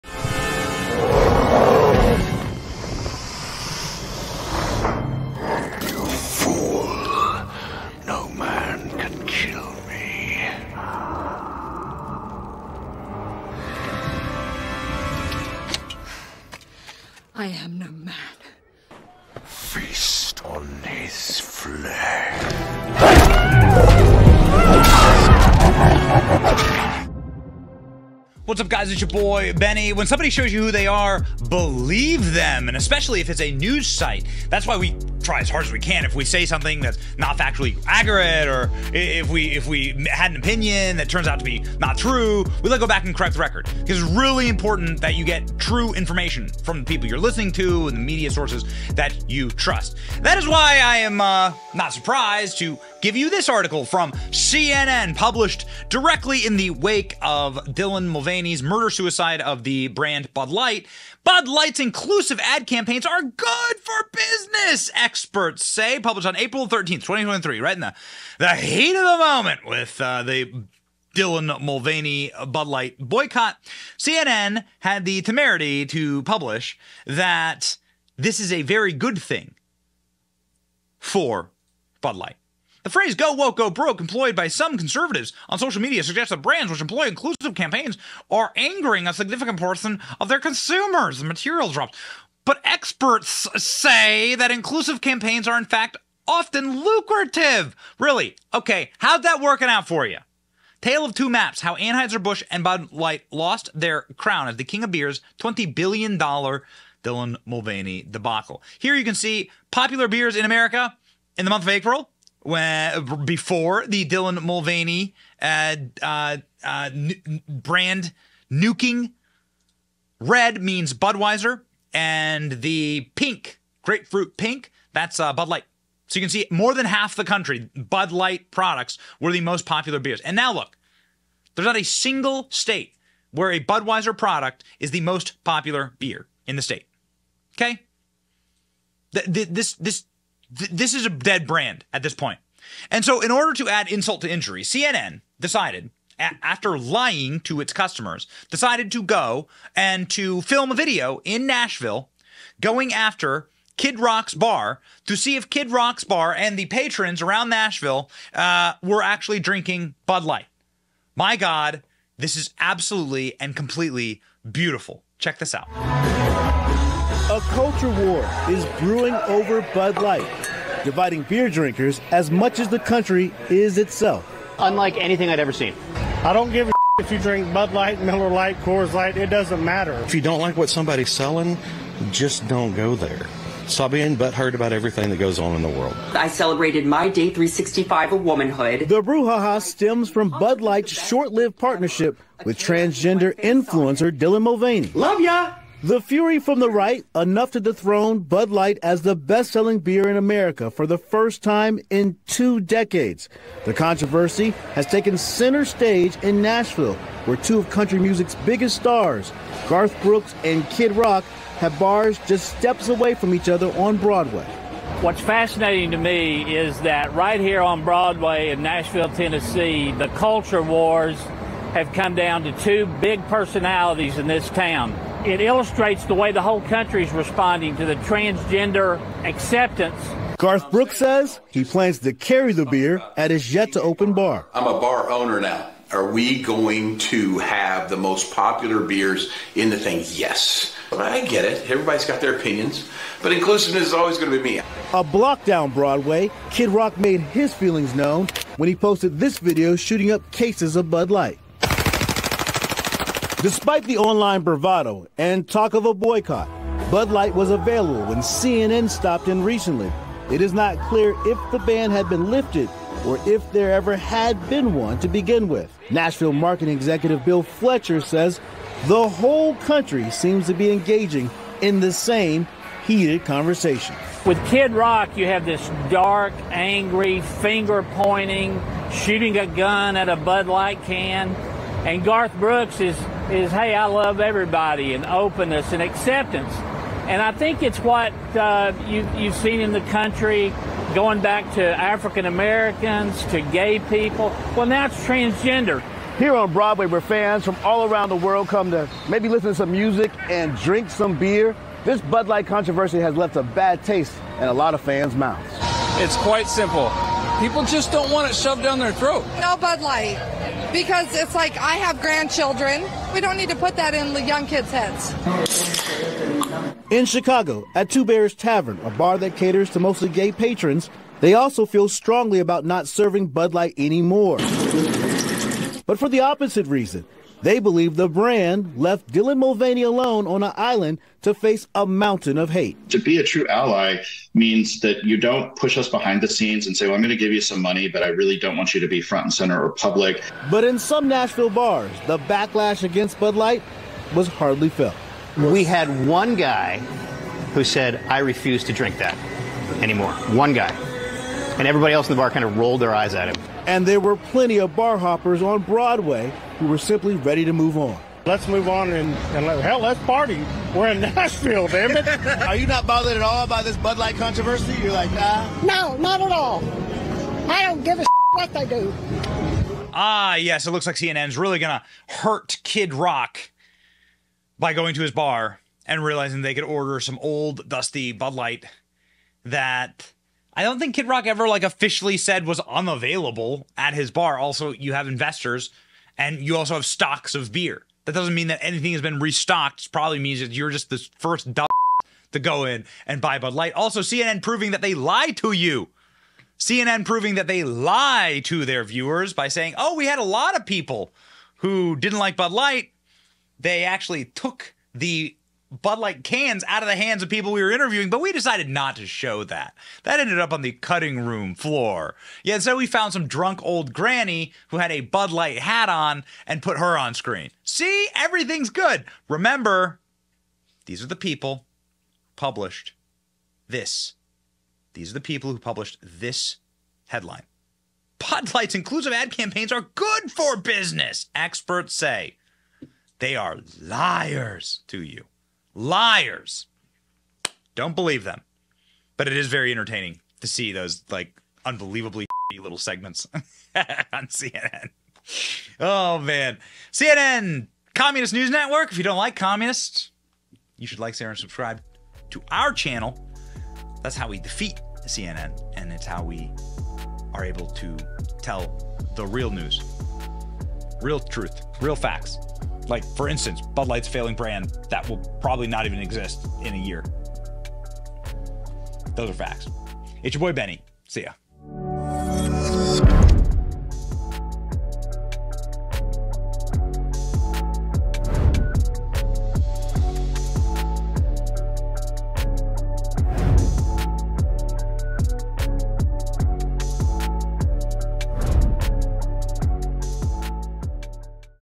CNN Walked Into Kid Rock’s Bar To Buy Bud Light | Guess What Happened Next...
Prepare for an AMAZING story that's SO OVER the top, you won't believe it! In this video, we reveal what happened when CNN walked into Kid Rock's bar with the simple intention of buying a Bud Light. The ensuing events are nothing short of SAVAGE, sparking a series of reactions that set the internet on FIRE.